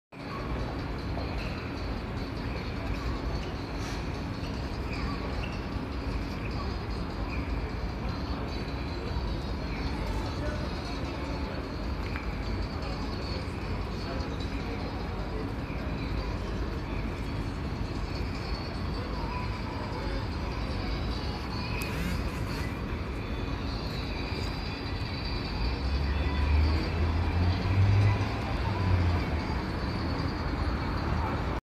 四条烏丸のバス停の前で大音量で音楽を聴いてる迷惑なヤツ
迷惑です